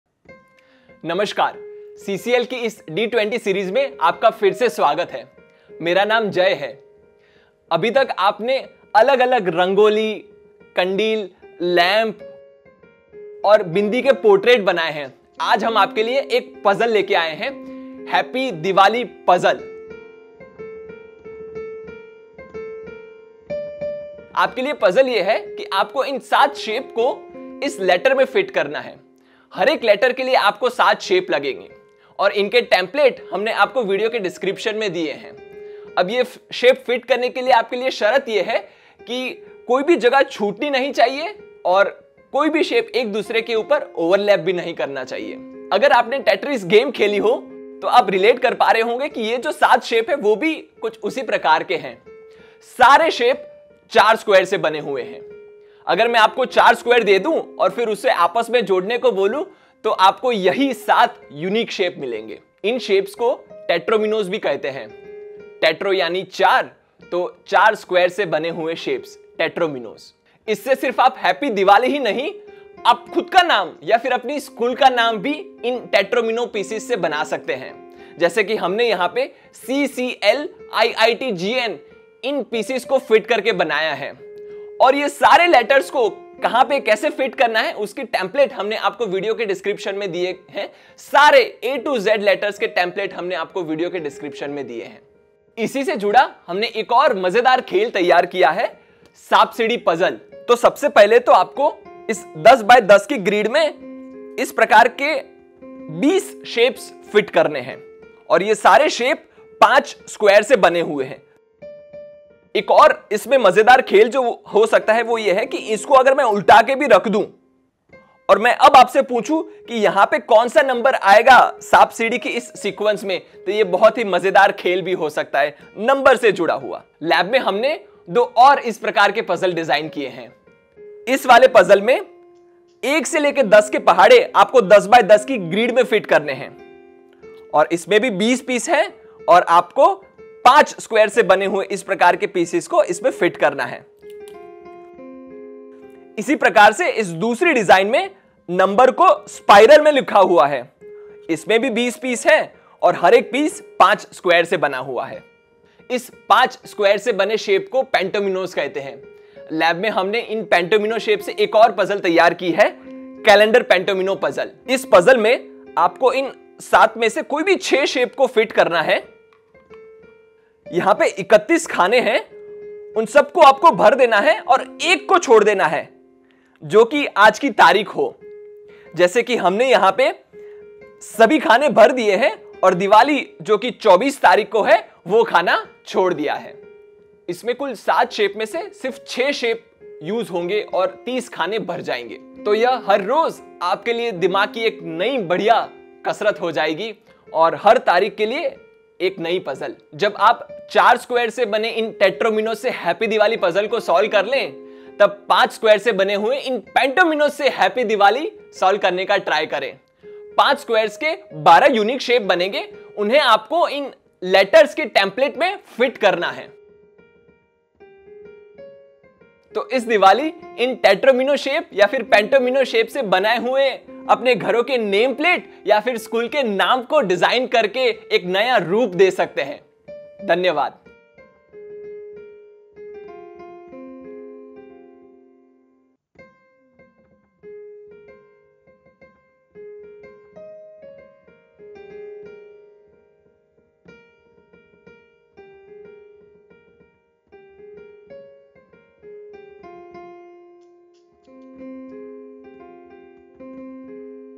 CCL D20 | HAPPY DIWALI Puzzle | Activity 19
Can you imagine making any word from specific seven shapes? These seven specific shapes are called Tetrominoes, in which shapes are composed of four squares. This time, CCL D20 continues with its second last, 19th amazing activity, creating 'Happy Diwali' Puzzle, presented by Mr. Jay Thakkar. This Diwali, let's do something interesting in a scientific way.

About Us